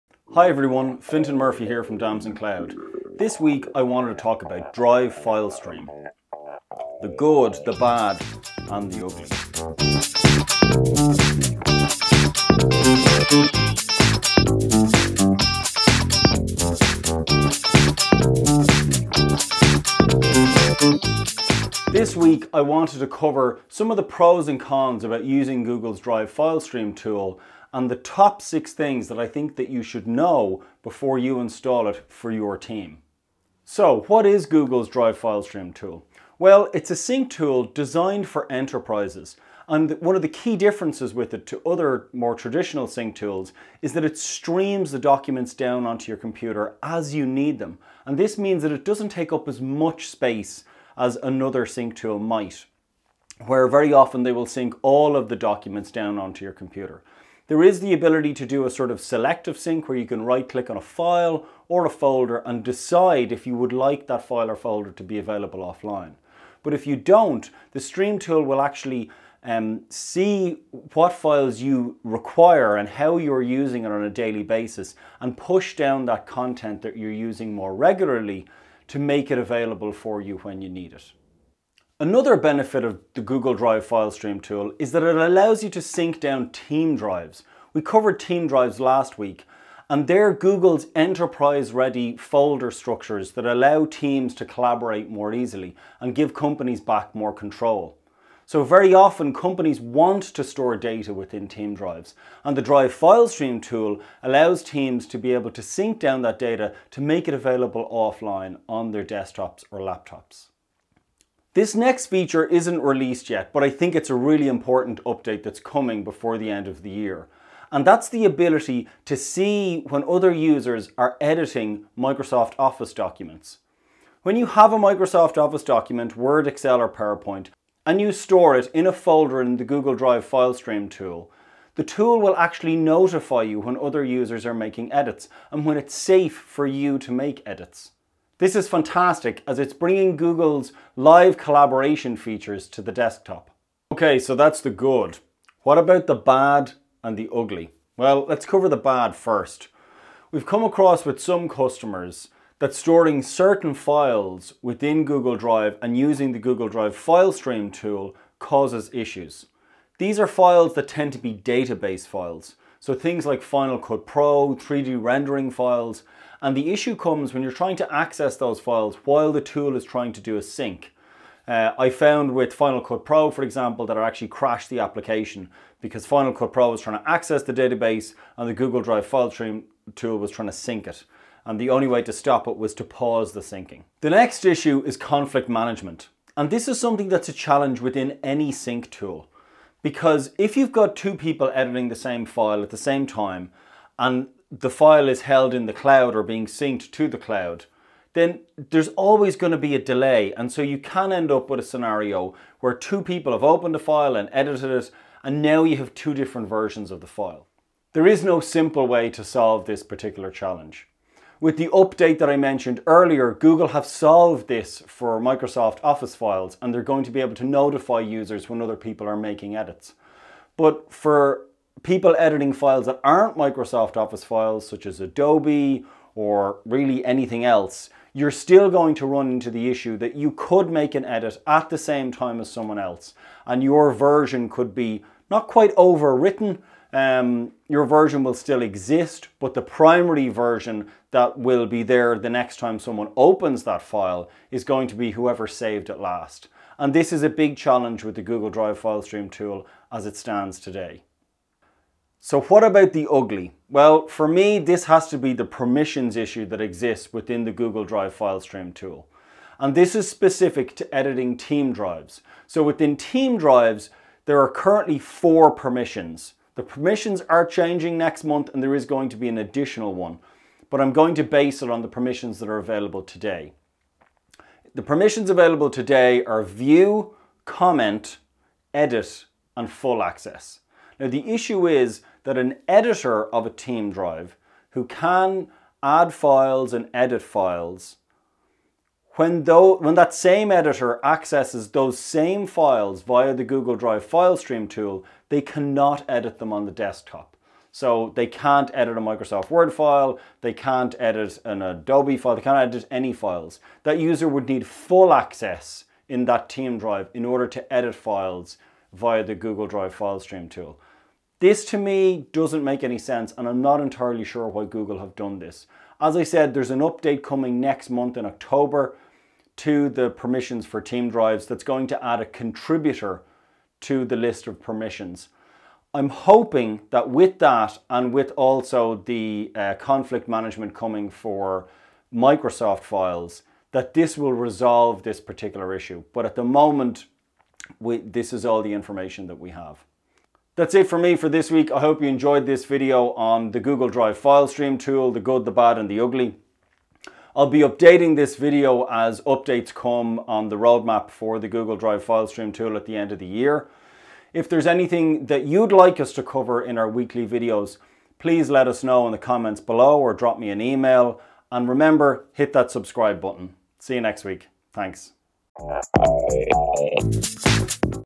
Google Drive File Stream tool - The Good, the Bad and the Ugly
This video covers the top 6 things you need to know before using the Google Drive file stream tool
We cover 3 pros and 3 cons.